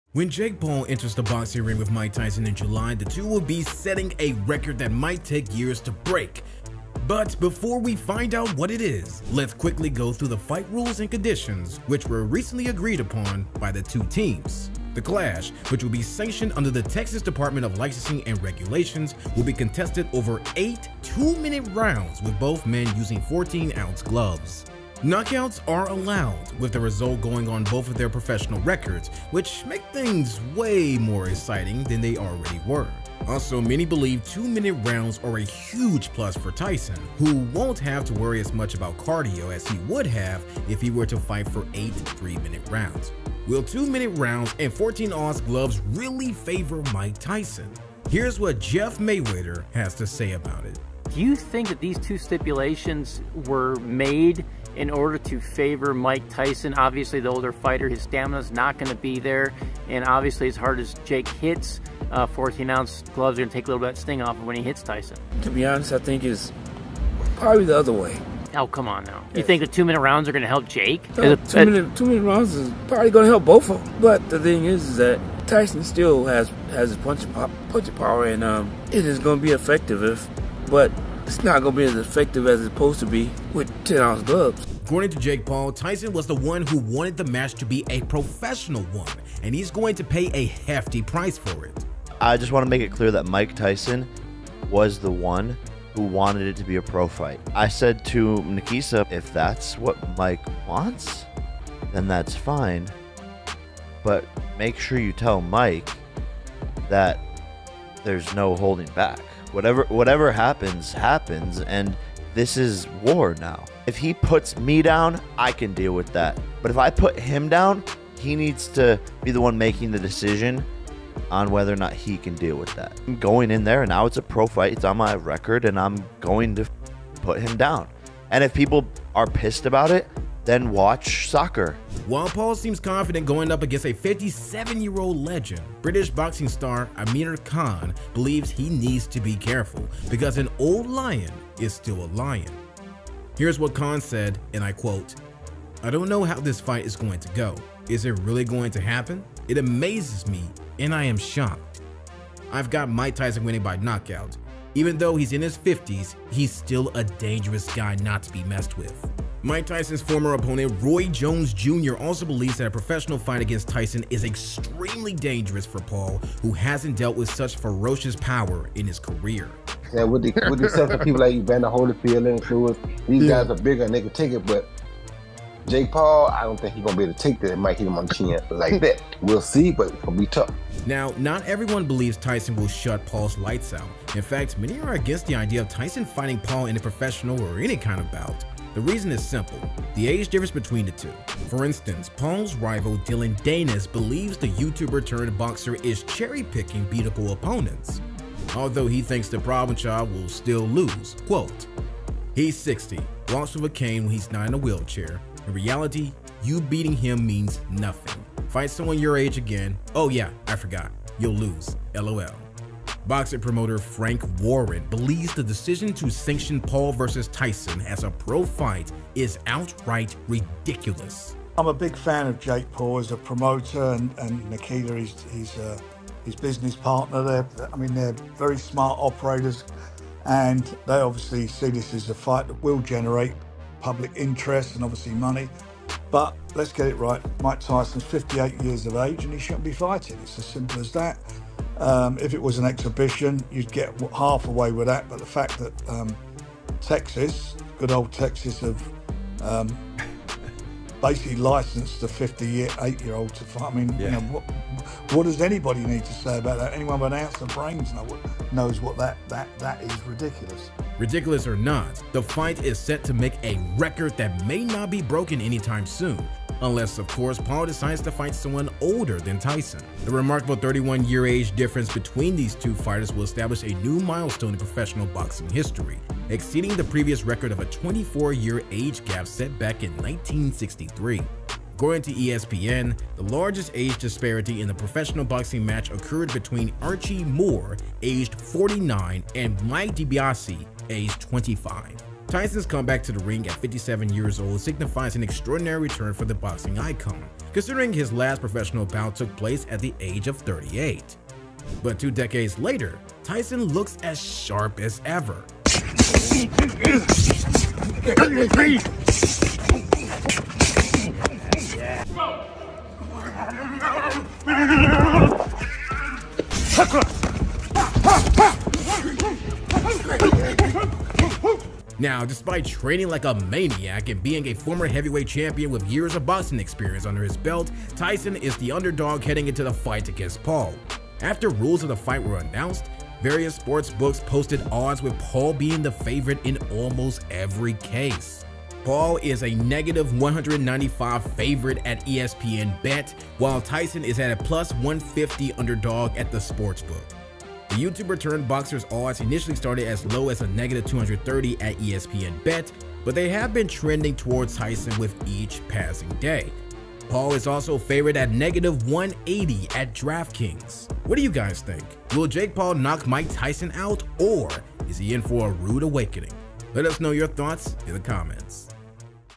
UPDATE! New Fight Rules For Tyson vs Paul
Shocking update on the fight rules for Paul vs Tyson. Watch the video to find out what the new guideless for this `proffesional bout will consist of. Who will benefit more from it, Paul or Tyson?